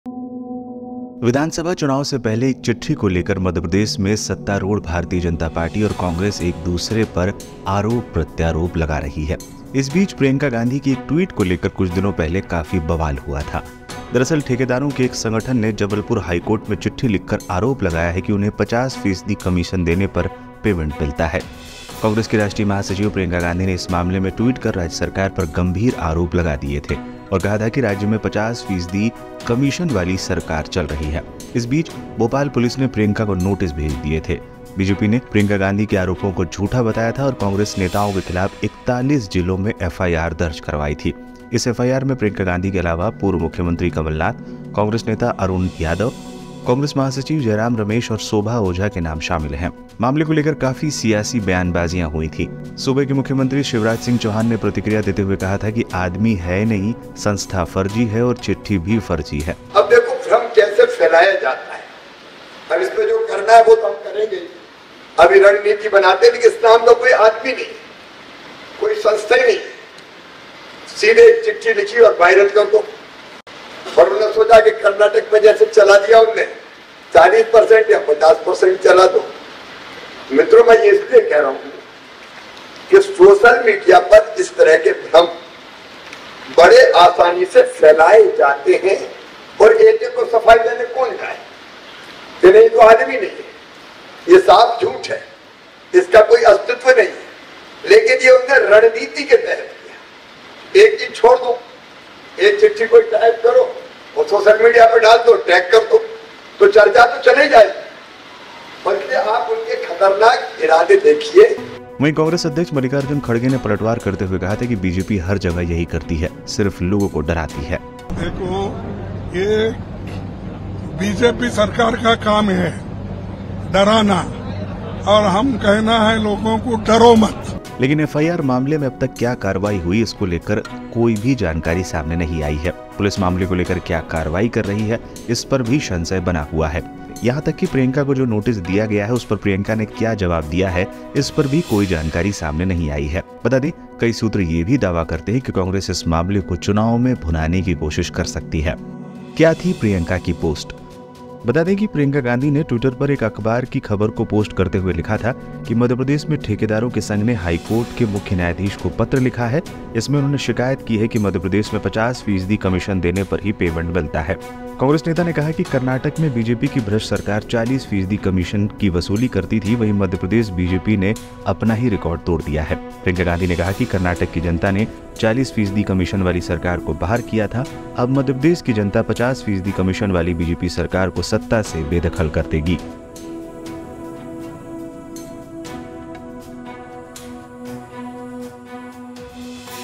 FIR Against Priyanka Gandhi: प्रियंका पर दर्ज हुई FIR में अब तक क्या हुई कार्रवाई | MP News
मध्य प्रदेश में ठेकेदारों के एक संगठन ने जबलपुर हाई कोर्ट को चिट्ठी लिखकर आरोप लगाया है कि उन्हें 50 फीसदी कमीशन देने पर पेमेंट मिलता है। कांग्रेस की राष्ट्रीय महासचिव प्रियंका गांधी ने इस मामले में ट्वीट कर राज्य सरकार पर गंभीर आरोप लगाए थे। जिसके बाद बीजेपी ने प्रियंका गांधी के आरोपों को झूठा बताते हुए उनके खिलाफ 41 जिलों में एफआईआर दर्ज करवाई थी। FIR मामले में अब तक क्या कार्रवाई हुई इसको लेकर कोई भी जानकारी सामने नहीं आई है।